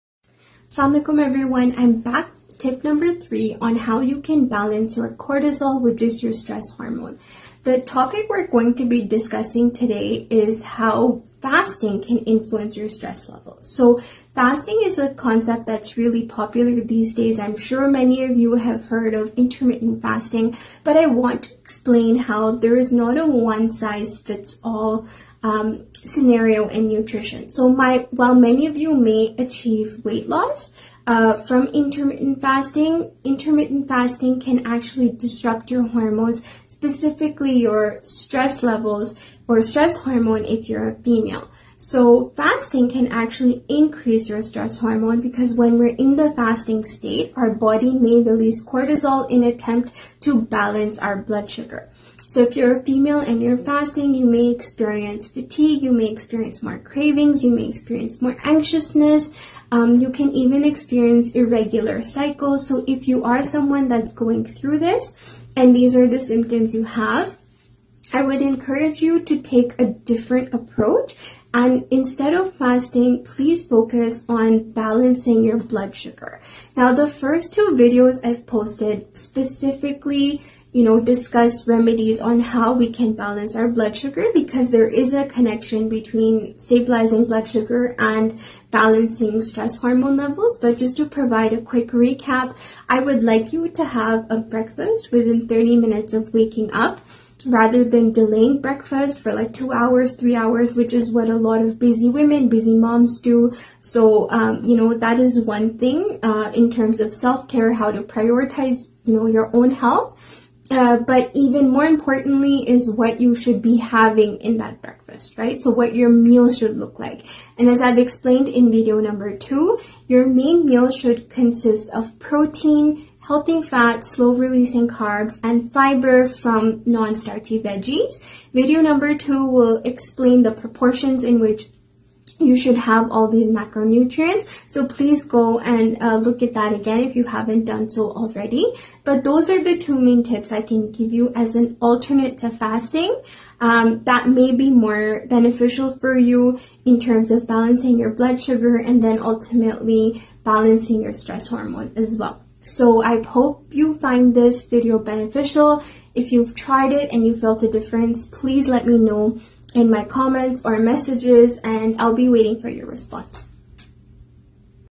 Can Fasting Raise Your Cortisol? What Every Woman Needs to Know
Is intermittent fasting doing more harm than good? In this video, I break down how fasting can increase cortisol levels—especially in women—and what that means for your energy, mood, and metabolism.

As a certified Holistic Nutritional Consultant, I’ll help you understand:

The science behind fasting and cortisol

Why stress hormones spike during prolonged fasting

Who should and shouldn’t fast regularly

Smarter alternatives to support hormone health

💚 If you’ve been feeling anxious, wired-but-tired, or struggling with belly fat—this video is for you.